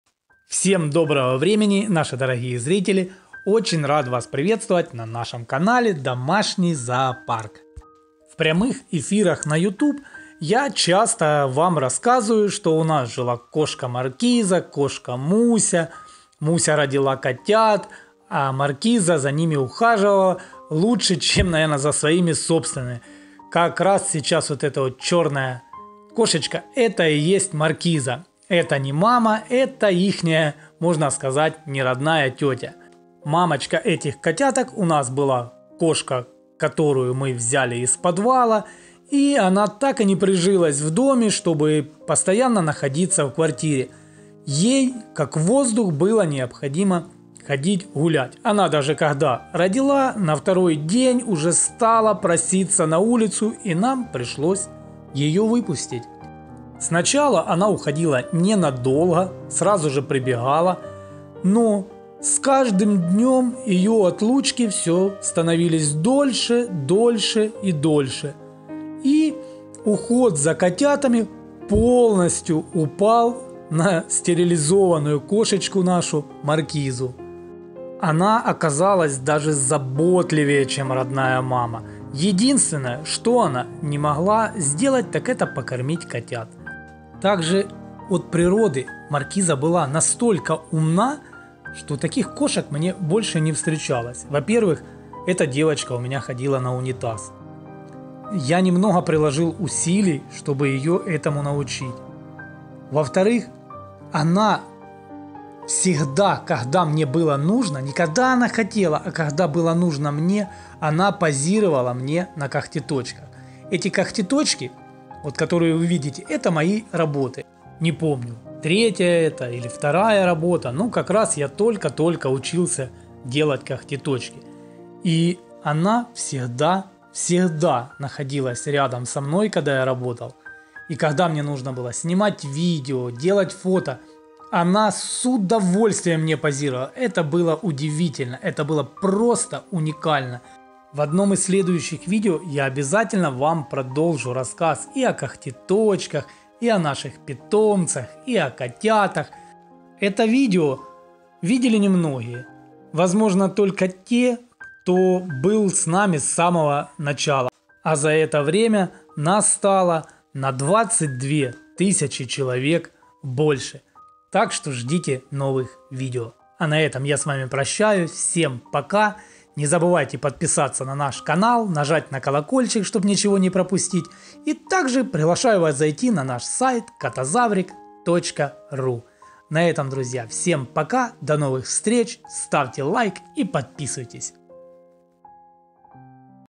Как наша кошка стала няней, для чужих котят
Наша Маркиза была необыкновенная кошка!